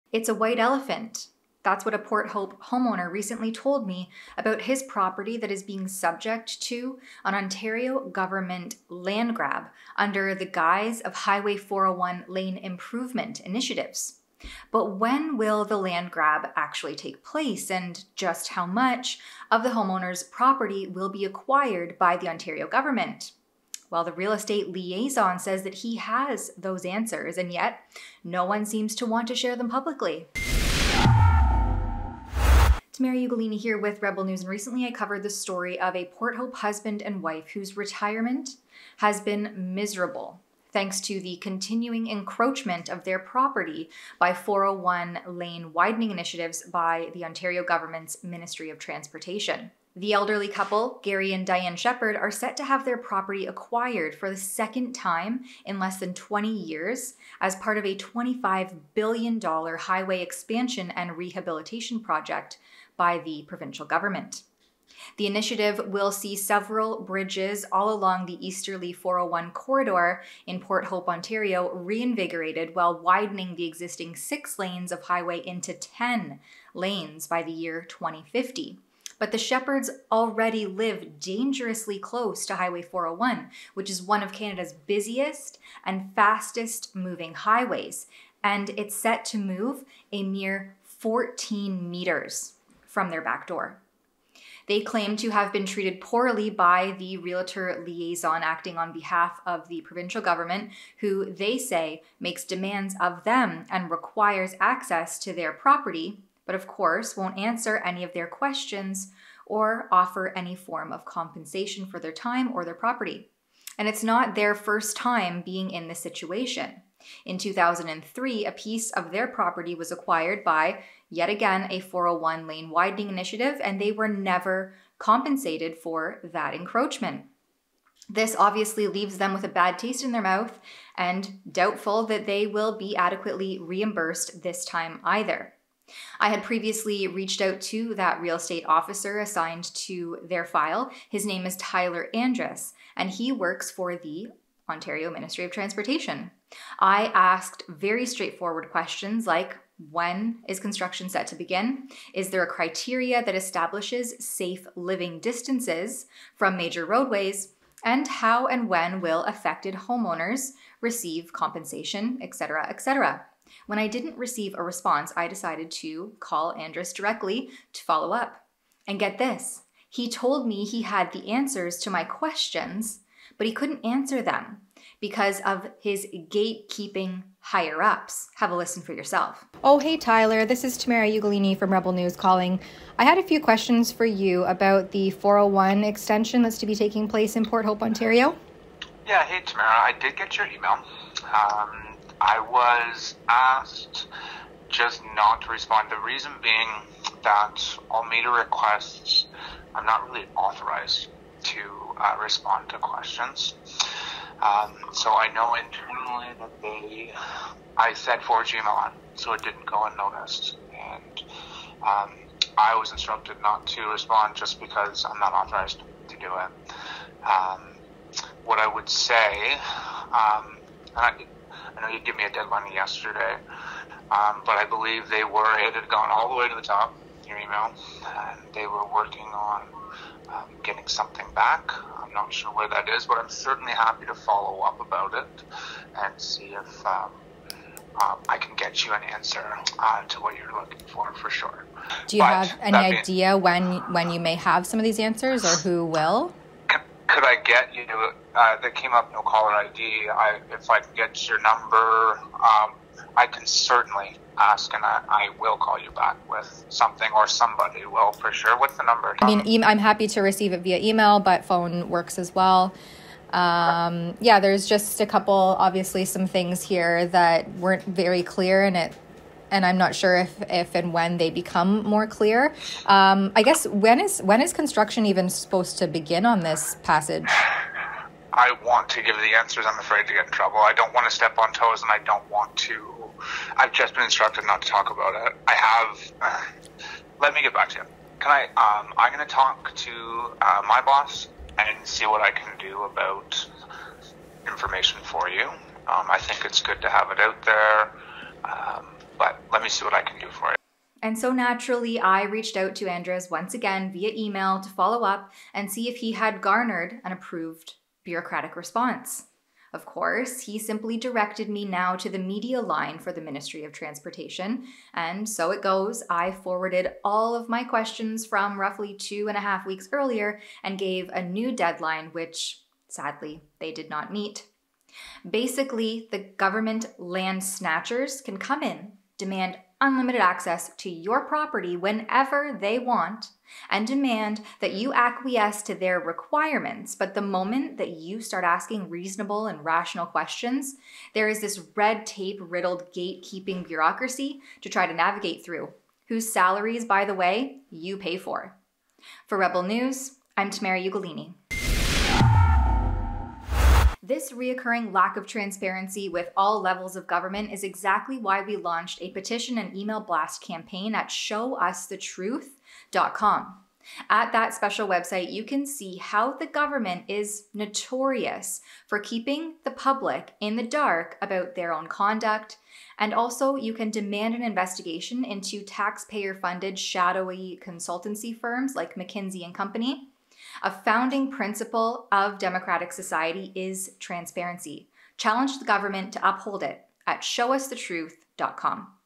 Ontario Government Snatches Land From Retired Couple
Rebel News' Tamara Ugolini provides an update on the Port Hope, Ontario retired couple whose land is being snatched up by the Ontario government.

for more great Rebel content.

Make sure to follow Rebel News' main channel on social media too!